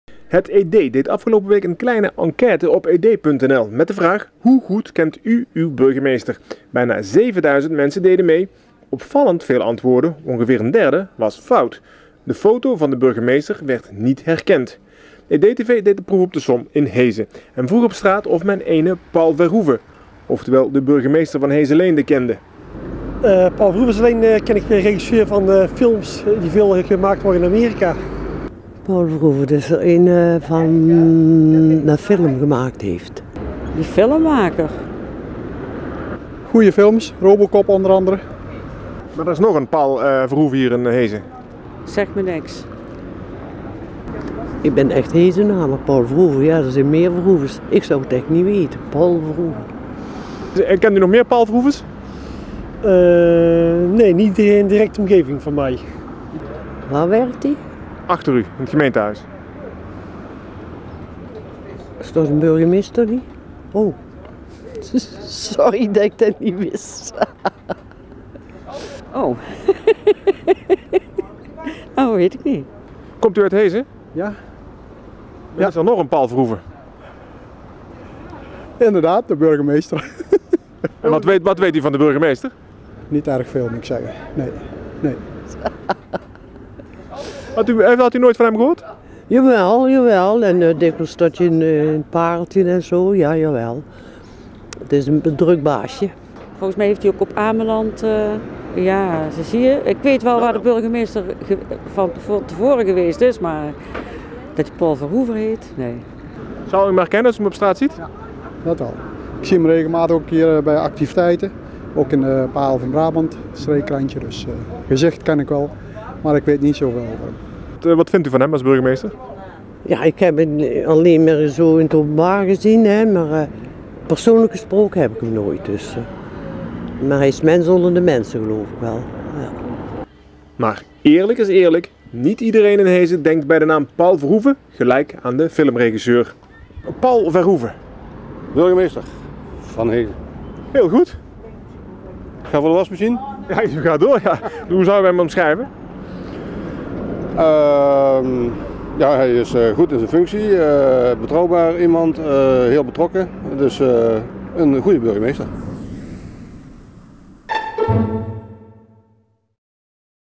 Heeze: Paul Verhoeven?
Het ED hield afgelopen week een enquete op ed.nl met de vraag: 'Hoe goed kent u uw burgemeester?' Bijna 7000 mensen deden mee. EDtv deed de proef op de som in Heeze en vroeg op straat of men ene Paul Verhoeven –oftewel de burgemeester van Heeze-Leende- kende